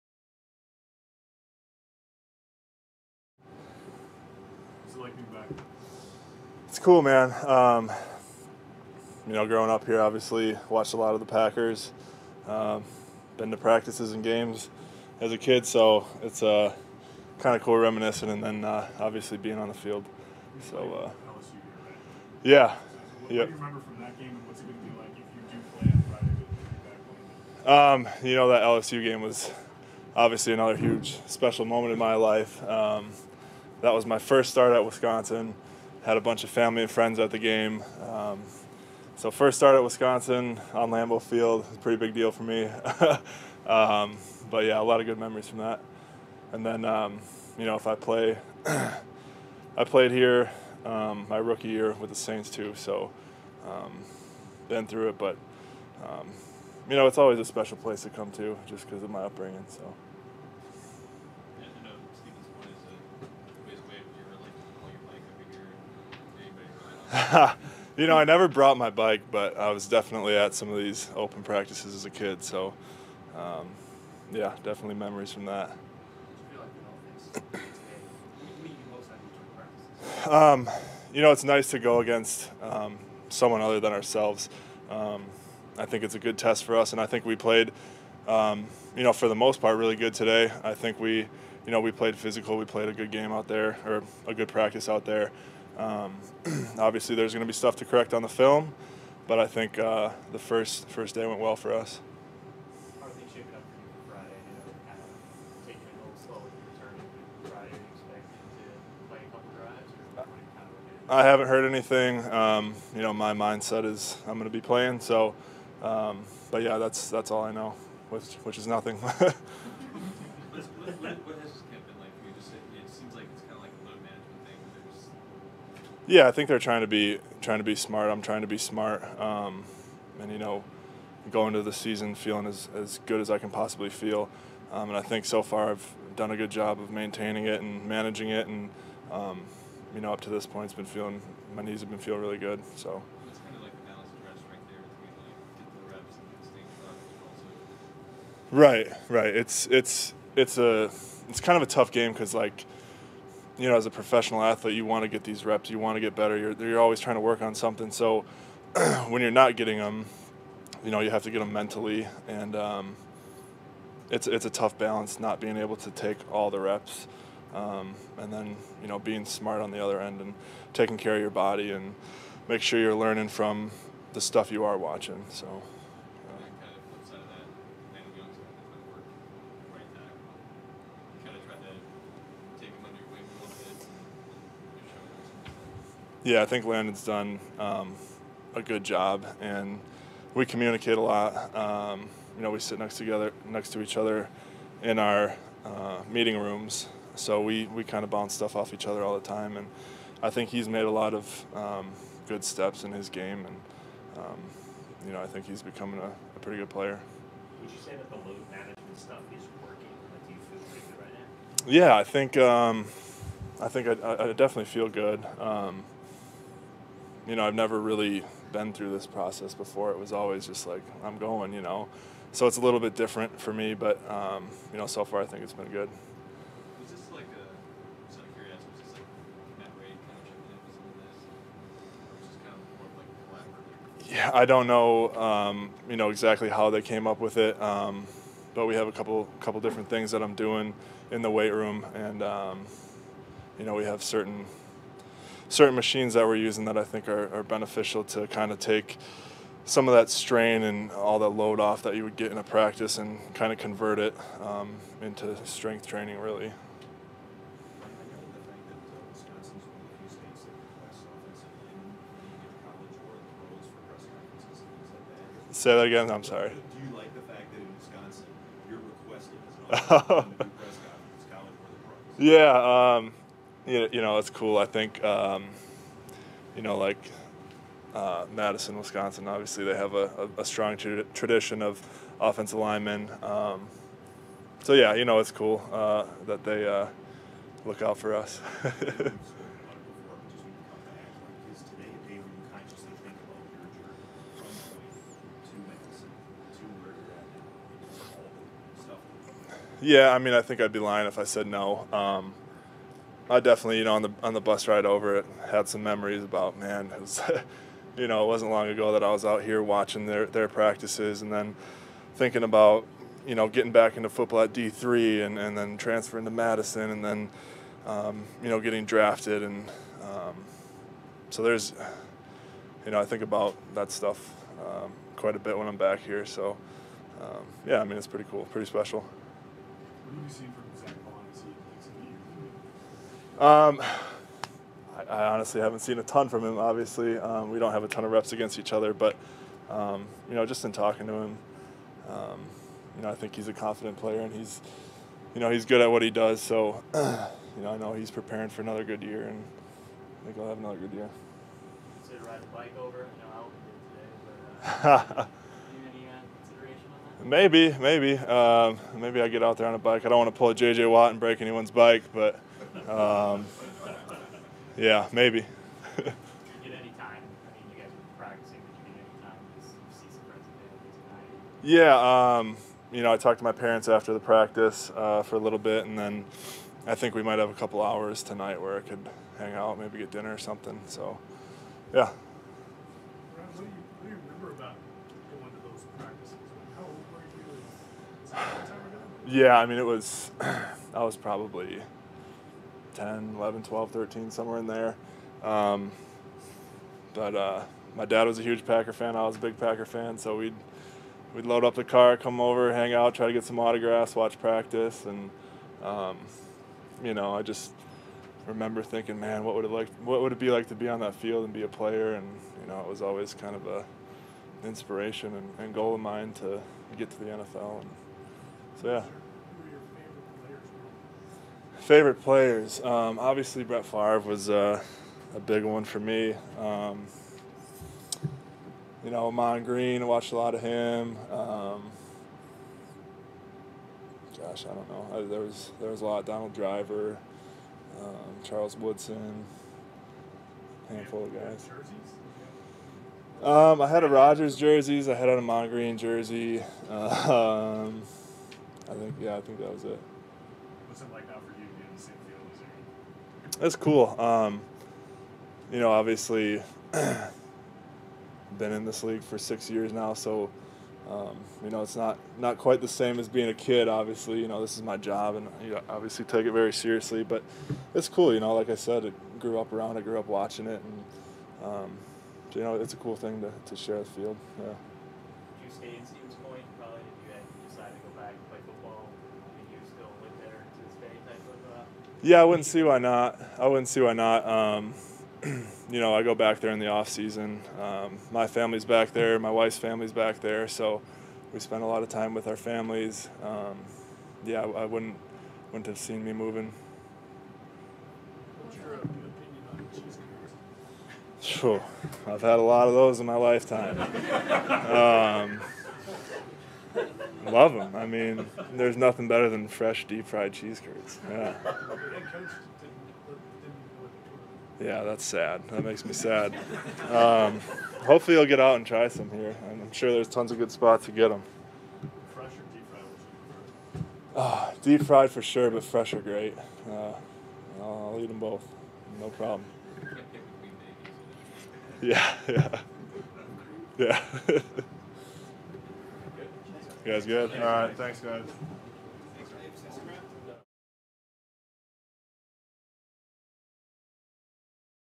Ryan Ramczyk on Return to Wisconsin for Preseason | Saints Training Camp 2022
New Orleans Saints right tackle Ryan Ramczyk post-practice interview from Saints Training Camp 2022 as they finish their first joint practice with the Green Bay Packers on August 16, 2022 ahead of their game in Week 2 of the 2022 NFL Preseason.